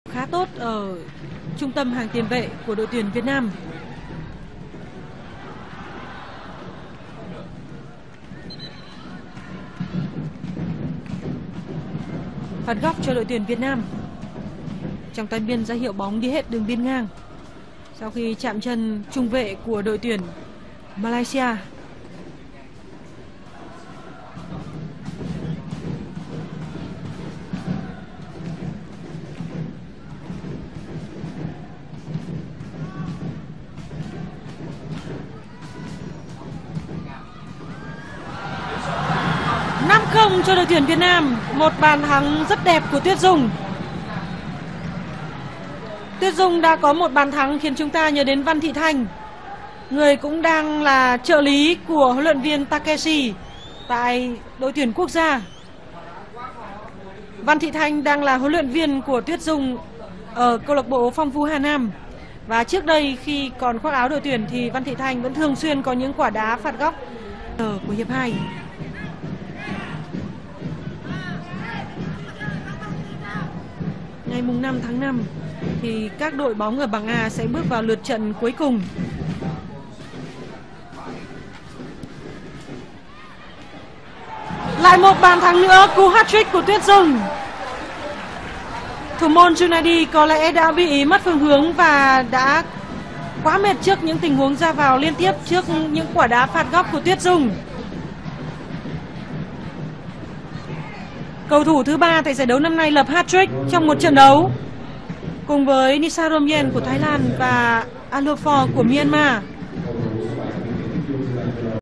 SIÊU PHẨM của Tuyết Dung | Sút thẳng từ chấm phạt góc vào gôn 2 lần trong 1 trận.
Trong trận gặp ĐT Nữ Malaysia, truyền nhân của Văn Thị Thanh là tiền vệ trẻ Nguyễn Thị Tuyết Dung đã lập nên một kỷ lục vô tiền khoáng hậu: ghi bàn từ chấm phát góc 2 lần trong một trận đấu, bằng 2 chân khác nhau từ 2 góc khác nhau.!!!